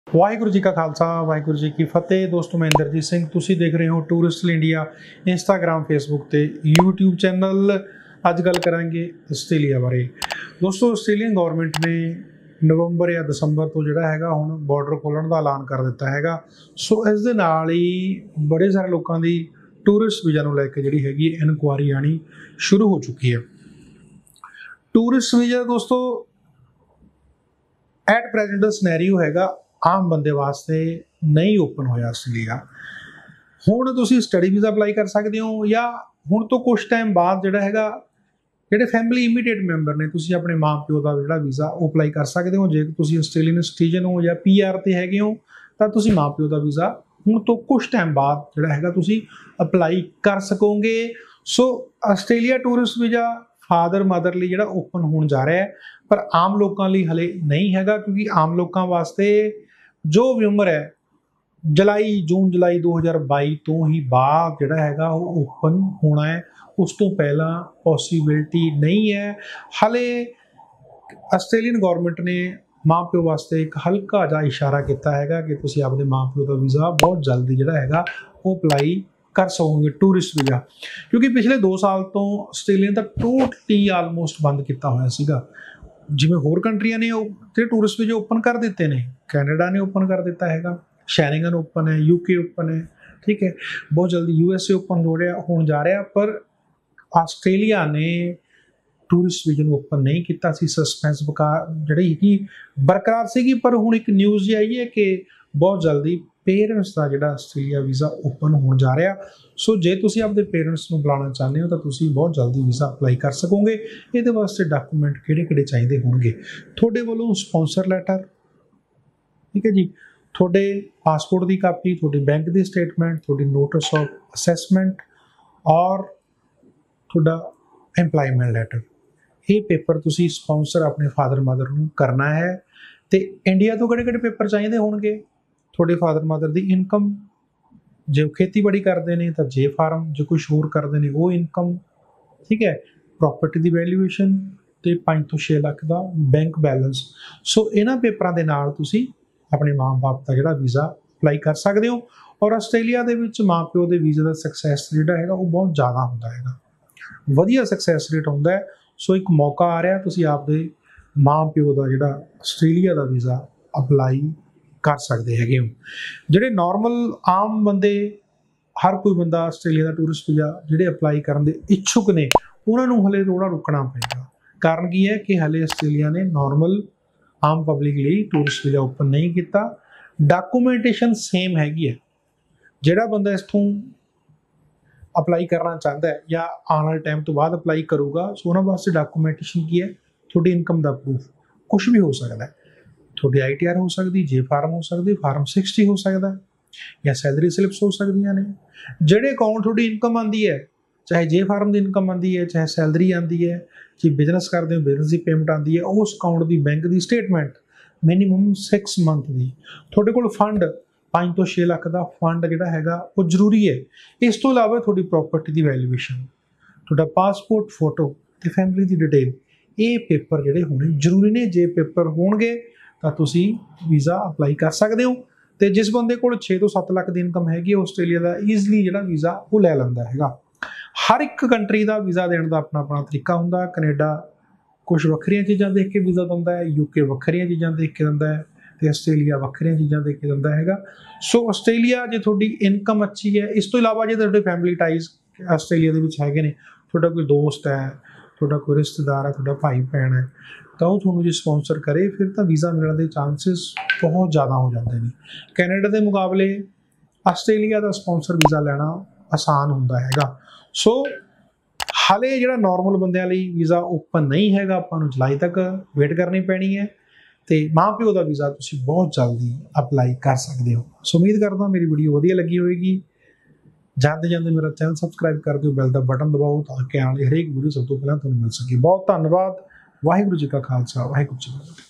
Australia Tourist Visa Latest Update 2021 ! Tourist visa for Australia from India 2021!
Australia Tourist visa  Start! Tourist visa for Australia from India 2021
Hello friends today's title of my video is Tourist visa for Australia from India 2021! Friends Australia has closed its border due to covid and friends there is no update yet as of now when Australia will open its border friends will definitely give the video till the last for complete information about Australia!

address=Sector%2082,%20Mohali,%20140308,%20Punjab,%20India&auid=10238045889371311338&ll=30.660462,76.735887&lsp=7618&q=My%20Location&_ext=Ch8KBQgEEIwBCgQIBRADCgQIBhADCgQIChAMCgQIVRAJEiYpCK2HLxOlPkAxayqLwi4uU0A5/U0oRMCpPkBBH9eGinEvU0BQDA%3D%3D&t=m